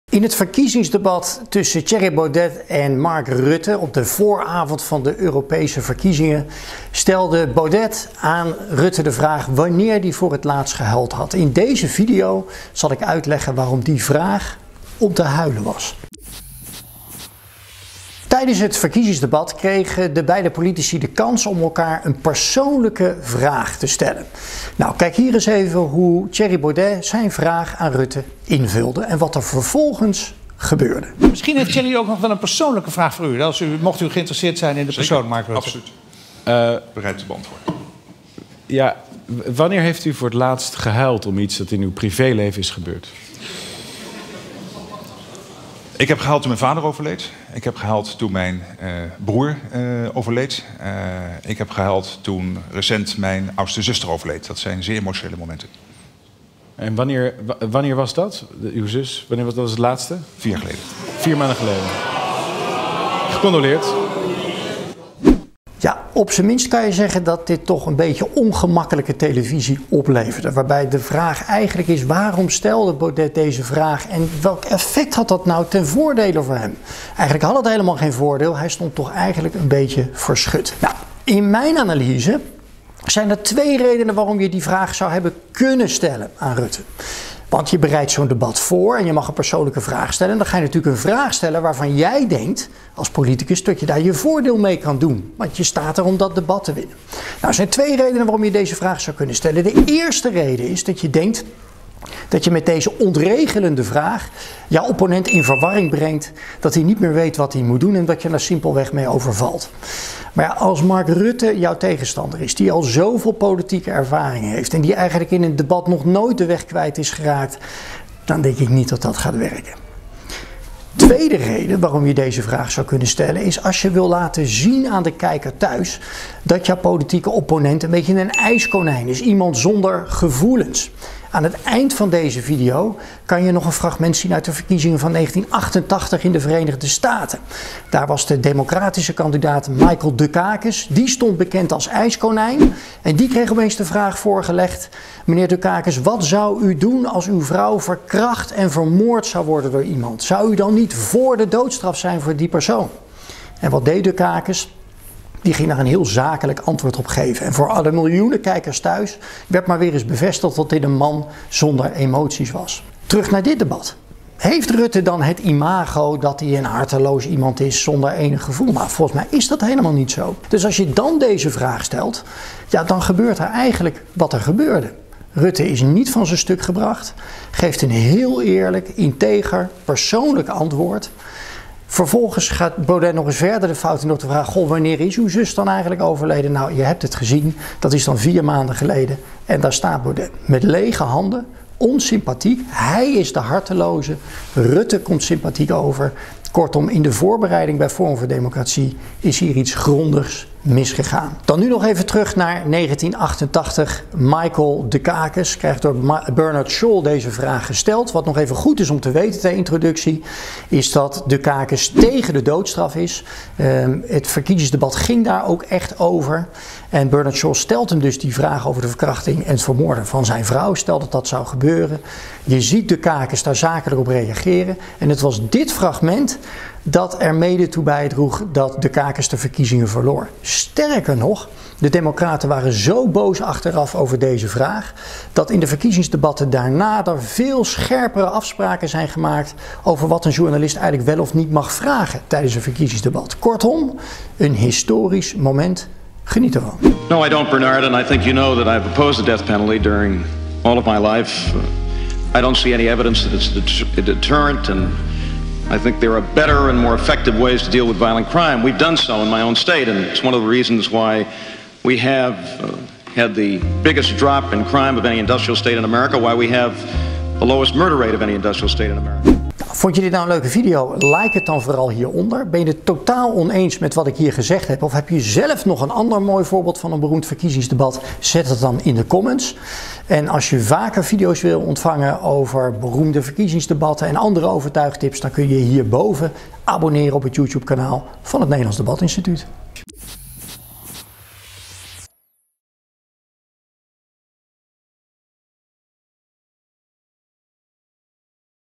Waarom de vraag van Baudet om te huilen was | IN HET NIEUWS
Baudet's vraag aan Rutte tijdens het verkiezingsdebat was om te huilen. Waarom? Dat legt Roderik van Grieken uit in deze video.